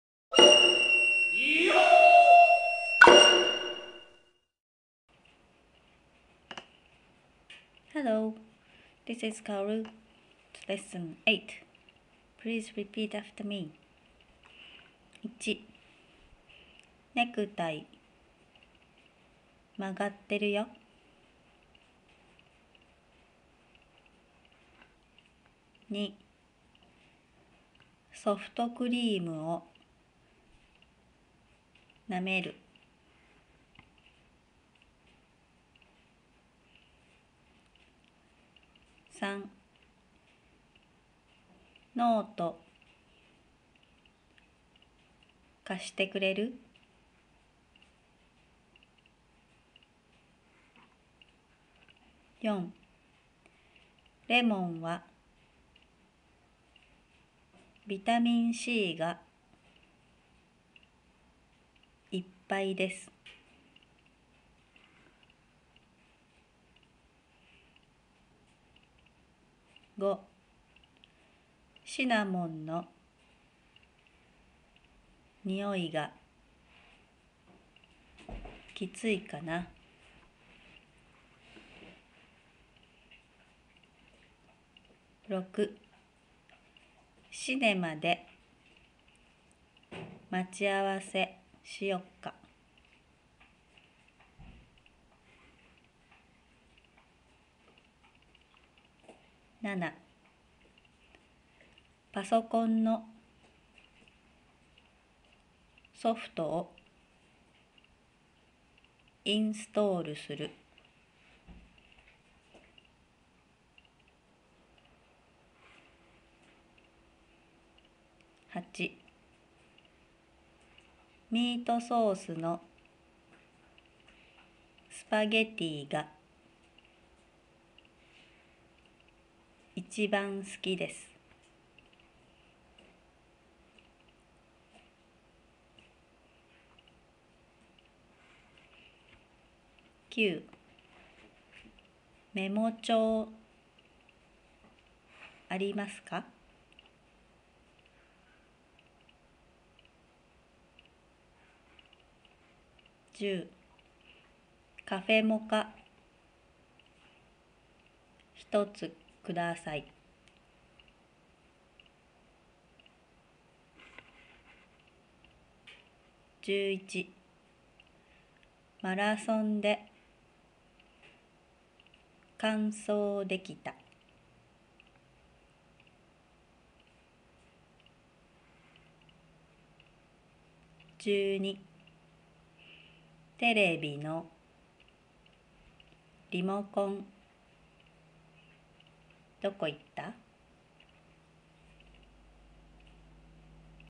Learn Katakana - Reading 8
Learn to read Katakana with these screen capture videos.